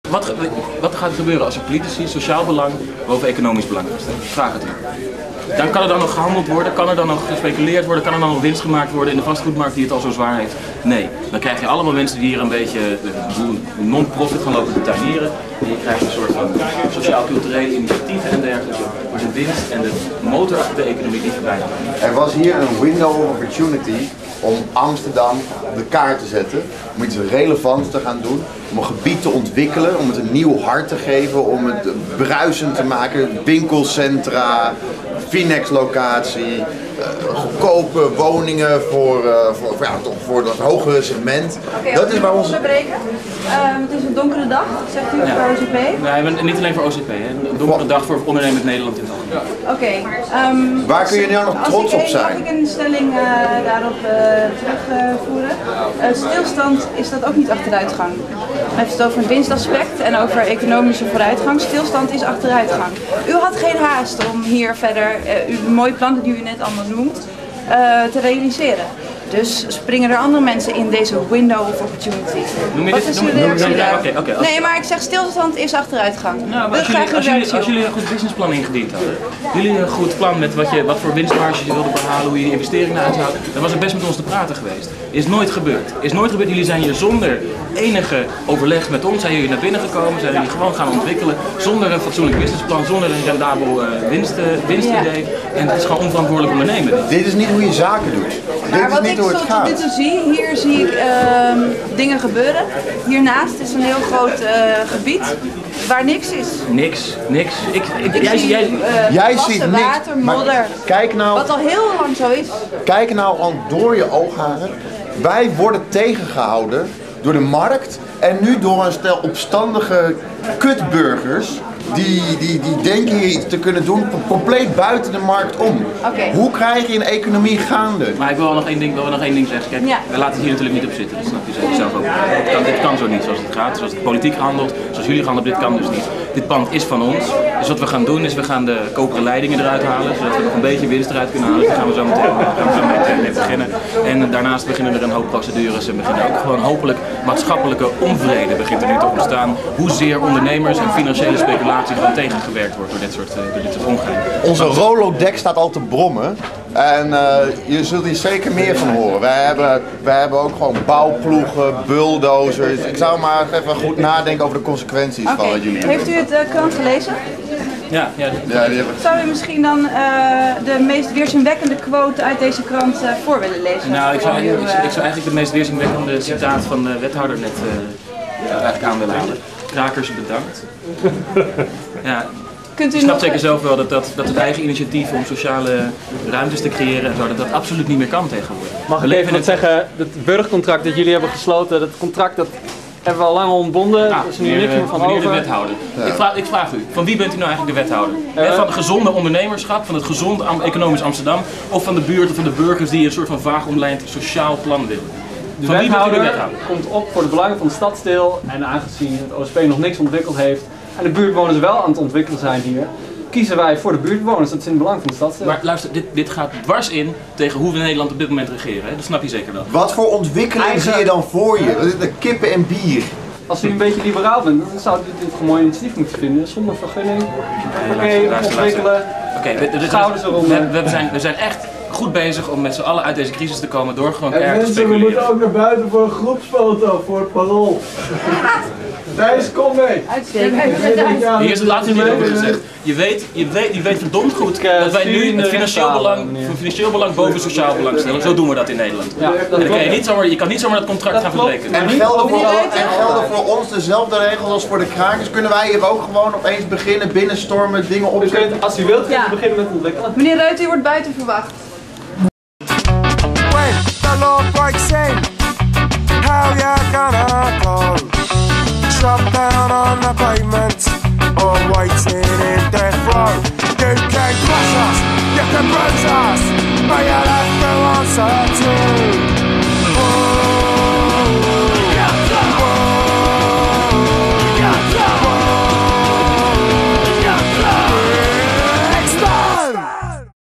Discussion with OCP at the Valreep
Krakers Valreep nemen stadsdeel op de hak
De vrijwilligers van buurtcentrum De Valreep in Amsterdam Oost hebben een satirische versie van de stadsdeelkrant verspreid. Hierin wordt melding gemaakt van de overdracht van het voormalige dierenasiel aan de krakers.
De persiflage op de stadsdeelkrant van Amsterdam Oost werd woensdag huis-aan-huis verspreid in de wijk Oosterpoort. Er wordt onder meer melding gemaakt van de feestelijke overdracht van het gekraakte dierenasiel aan de Polderweg, nu nog eigendom van het stadsdeel, aan stichting De Valreep, die sinds afgelopen zomer het gebouw in gebruik heeft. Deze overdracht, die zaterdag plaats moet vinden, wordt tevens op de website van De Valreep vermeld.
Direct na publicatie van de persiflage heeft het stadsdeel aangegeven niet de afzender te zijn en de mogelijk ontstane verwarring te betreuren. Het stadsdeel laat op z'n website weten dat ze de krant 'grappig en mooi gemaakt' vinden. 'U had vast al begrepen dat het stadsdeel echter niet de afzender is', schrijft Oost.
In een aanvullend persbericht, verspreid door de afdeling communicatie van stadsdeel Amsterdam Oost maar zeer waarschijnlijk ook nep, worden de toekomstperspectieven van buurtcentrum De Valreep toegelicht. "Projectontwikkelaar OCP heeft de afgelopen jaren, tegen de oorspronkelijke afspraken in, de wijk Oostpoort niet ontwikkeld. Er waren plannen om het voormalige dierenasiel in gebruik te nemen als verkoopkantoor maar de geplande woningen worden de komende jaren nog niet gerealiseerd", meldt het stadsdeel.
"Buurtbewoners hebben nu wél de ontwikkeling van het voormalige dierenasiel ter hand genomen. Ze bieden bovendien meer voor het erf dan OCP. En ten slotte past een maatschappelijke functie voor het dierenasiel uitstekend in de plannen om Oostpoort te ontwikkelen tot nieuw stedelijk centrum." Om deze redenen, en in goed overleg met het OCP, zou het stadsdeel bezig zijn de vrijwilligersinitiatieven blijvend een plek te geven in het voormalige dierenasiel.
In een echte uitgave van de stadsdeelkrant van september 2011 meldt de lokale politiek dat het merendeel van de fracties weliswaar positief staat voor het handhaven van een blijvend buurt-/activiteitencentrum in het monumentale gebouw, maar portefeuillehouder Wonen, Thijs Reuten, ziet geen mogelijkheden om het centrum te legaliseren. In 2012 wil OCP een verkoopkantoor in het pand openen. Het stadsdeel heeft met OCP de afspraak dat het het pand leeg oplevert, zodra er concrete bouwplannen liggen.